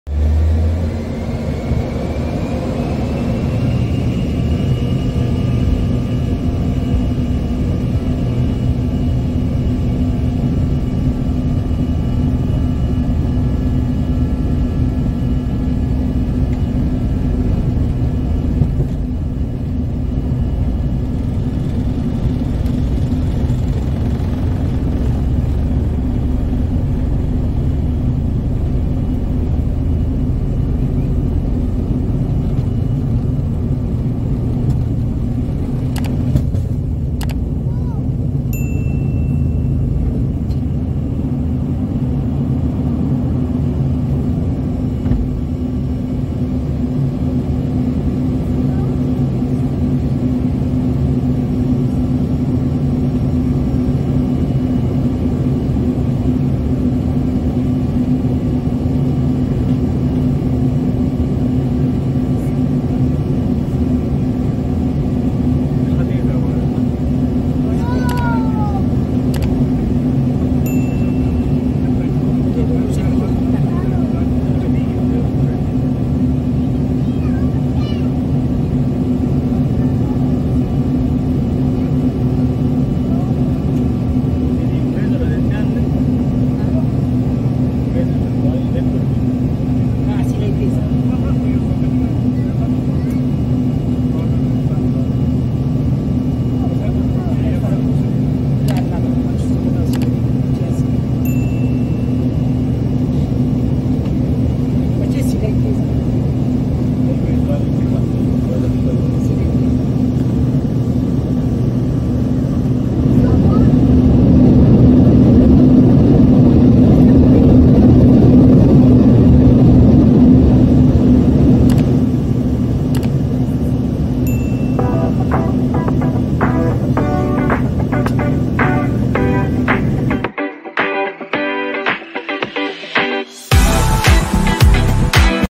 American Airlines Embraer ERJ-145 Takeoff at Toronto Pearson International Airport!
Being in a small plane, you experience more turbulence compared to a larger aircraft. The experience becomes even more intense during the landing phase, resembling the feeling of a roller coaster descending rapidly. The Embraer ERJ-145 planes operated by American Airlines are older models. Due to their smaller size, they have a quicker takeoff, necessitating a rapid decrease in altitude. As a first-time passenger on this aircraft, this aspect was particularly frightening. Additionally, the weather conditions further contributed to the turbulence.